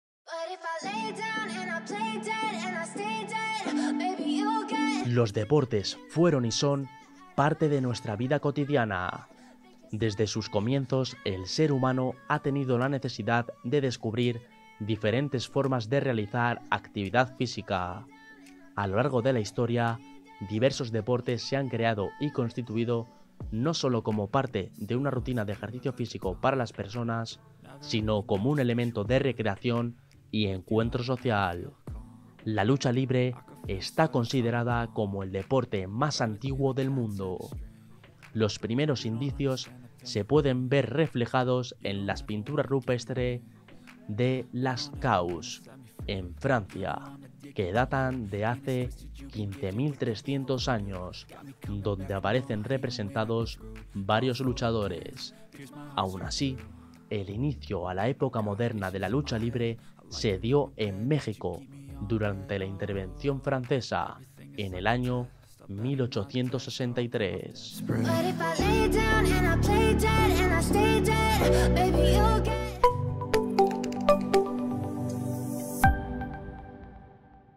La lucha libre, el deporte más antiguo del mundo
Los deportes fueron y son parte de nuestra vida cotidiana. Desde sus comienzos, el ser humano ha tenido la necesidad de descubrir diferentes formas de realizar actividad física. A lo largo de la historia, diversos deportes se han creado y constituido, no sólo como parte de una rutina de ejercicio físico para las personas, sino como un elemento de recreación y encuentro social. La lucha libre está considerada como el deporte más antiguo del mundo. Los primeros indicios se pueden ver reflejados en las pinturas rupestres de Lascaux, en Francia, que datan de hace 15.300 años, donde aparecen representados varios luchadores. Aún así, el inicio a la época moderna de la lucha libre, se dio en México durante la intervención francesa, en el año 1863.